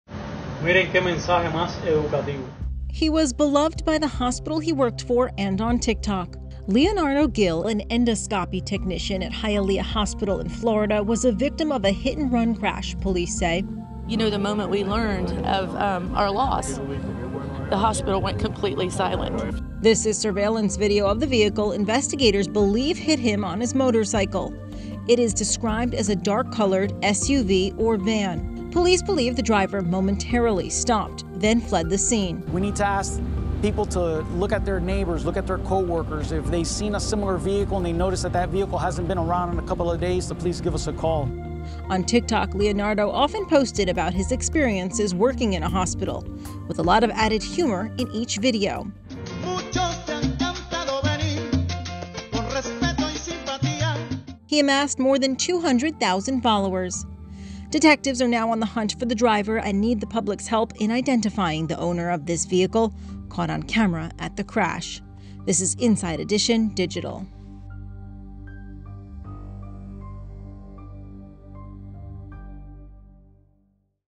Hospital Worker With 200,000 TikTok Followers Killed in Hit-and-Run Crash
Leonardo Gil, an endoscopy technician, was killed in a hit-and-run crash, police say. It happened in Hialeah, Florida. Gil was beloved by the hospital where he worked and on TikTok. He often posted about his experiences working in a hospital, along with some humor for comic relief. Surveillance video has been released of the vehicle police say was involved in the deadly crash. They need the public’s help in identifying the SUV or van and its driver.